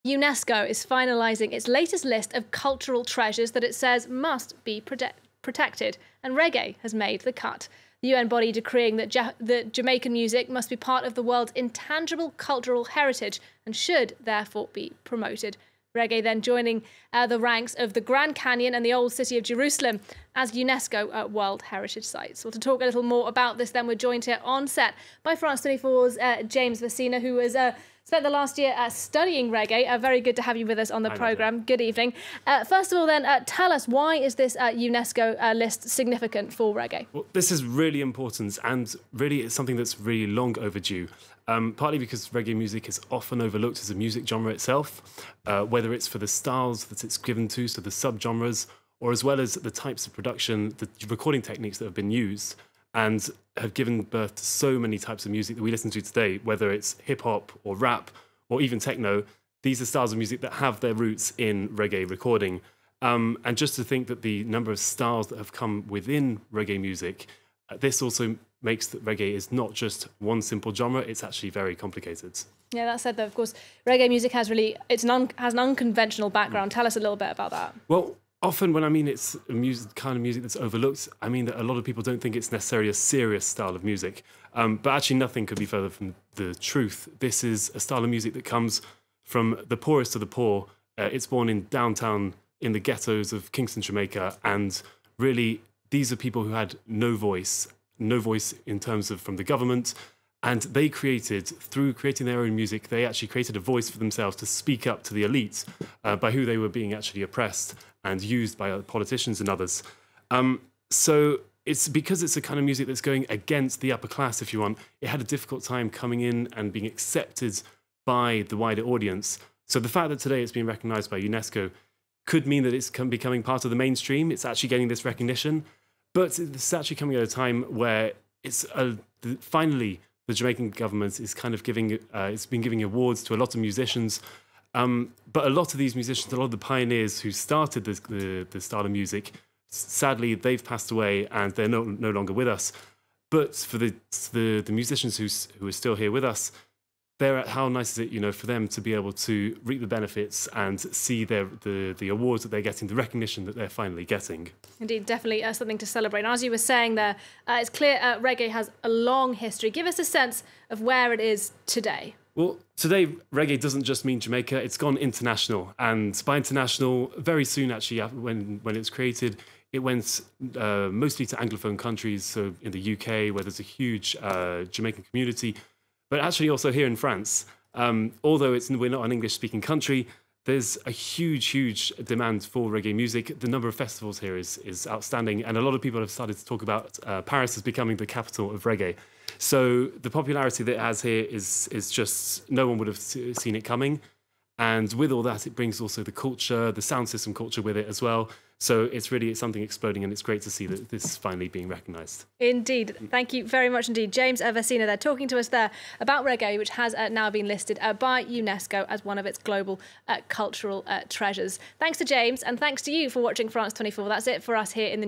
Reggae joins UNESCO cultural heritage: "It's really important and really long overdue"
Reggae music has found its groove on the United Nations' list of global cultural treasures. UNESCO, the world body's cultural and scientific agency, added the music genre to its collection of "intangible cultural heritage" deemed worthy of protection and promotion on Thursday. FRANCE24's James Vasina tells us more.